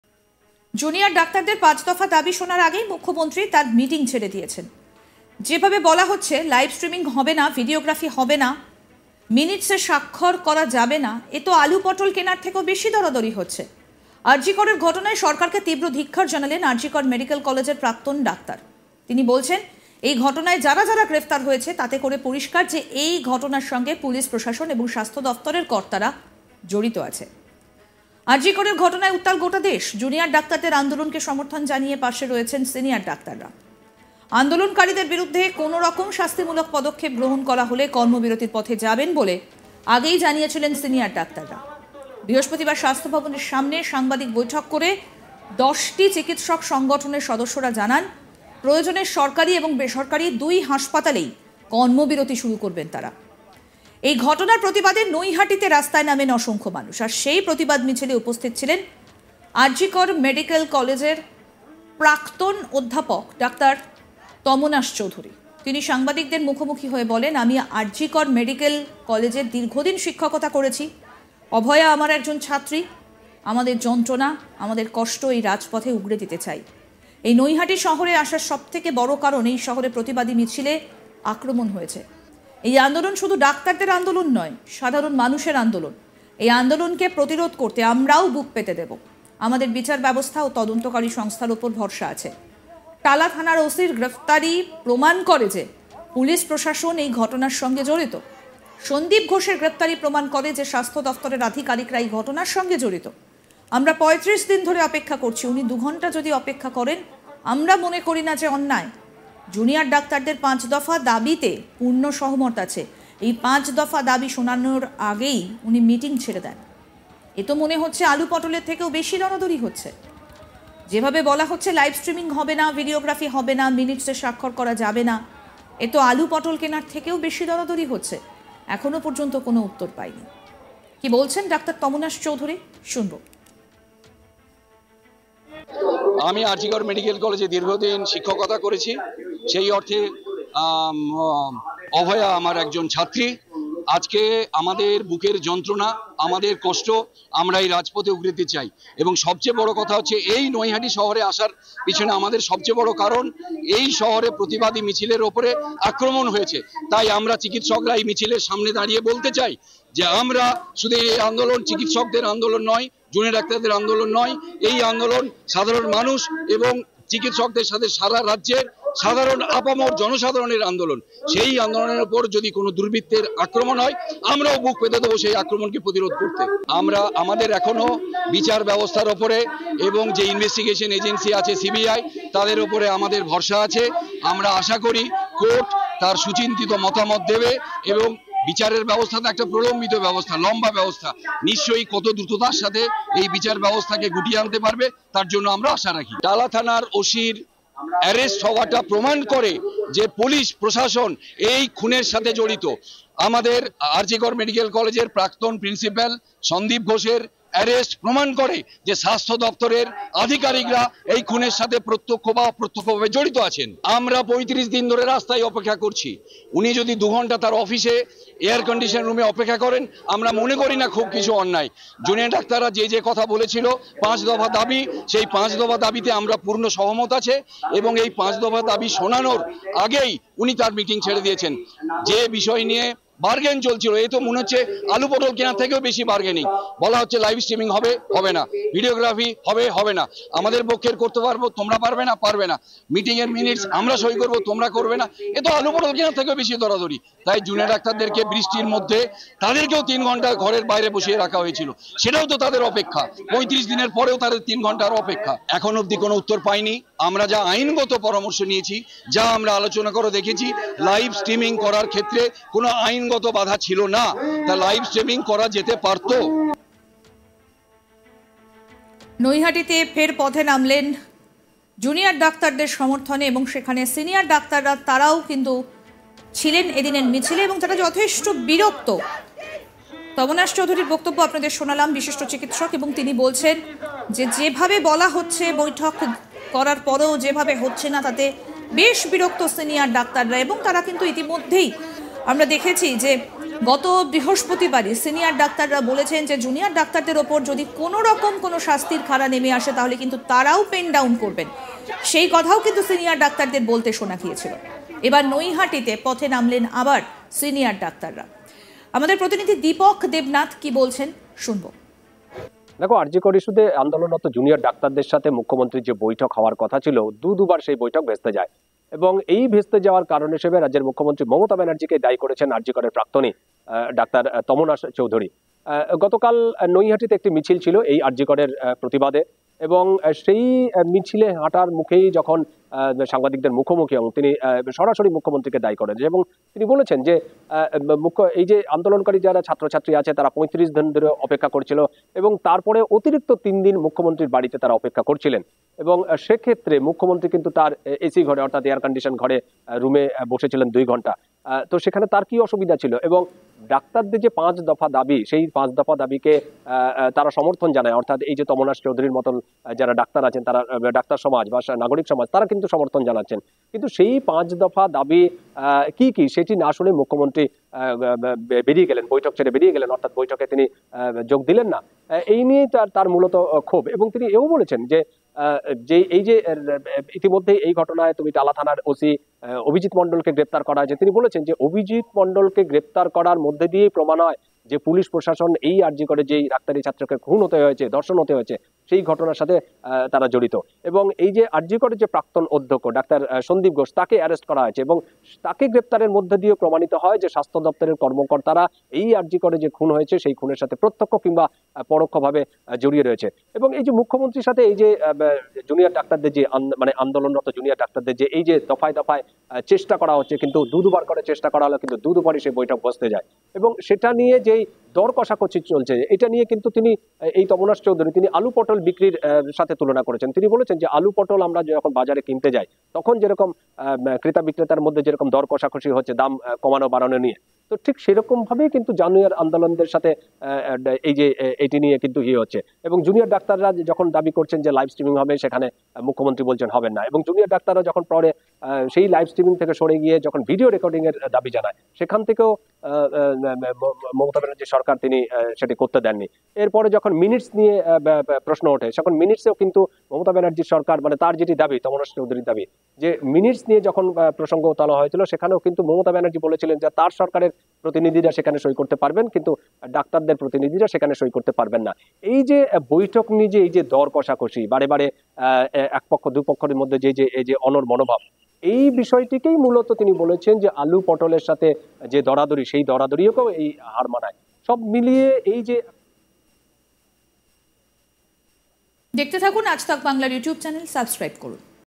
লাইভ স্ট্রিমিং জটিলতায় বিরক্ত সিনিয়র ডাক্তার' আলু,পটলের থেকেও বেশি দর হচ্ছে' | RG Kar Protest
জুনিয়ার ডাক্তারদের পাঁচ দফা দাবি শোনার আগেই মুখ্যমন্ত্রী তার মিটিং ছেড়ে দিয়েছেন। যেভাবে বলা হচ্ছে লাইভ স্ট্রিমিং হবে না, ভিডিওগ্রাফি হবে না, মিনিটসে স্বাক্ষর করা যাবে না- এতো আলু, পটল কেনার থেকেও বেশি দরাদরি হচ্ছে। আর জি করের ঘটনায় সরকারকে তীব্র ধিক্কার জানালেন আর জি কর মেডিক্যাল কলেজের প্রাক্তন ডাক্তার। তিনি বলছেন এই ঘটনায় যাঁরা যাঁরা গ্রেপ্তার হয়েছে তাতে করে পরিষ্কার যে এই ঘটনা সঙ্গে পুলিশ প্রশাসন ও স্বাস্থ্যদপ্তরের কর্তারা জড়িত আছে।